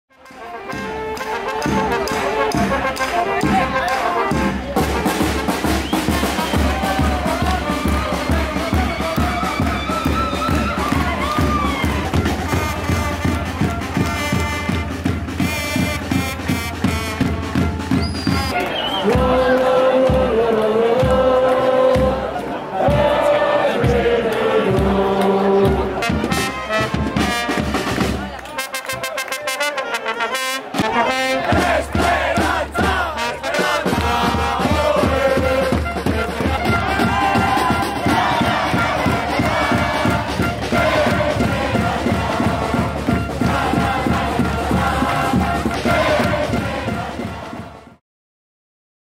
Herritarren berotasuna sari handiena
Kontxako Banderako bigarren estropada jokatu ostean, zazpigarren postua eskuratu du "Esperantza"k. Arraunlariak animatu asmoz, getariar zaletu ugari izan da Donostian.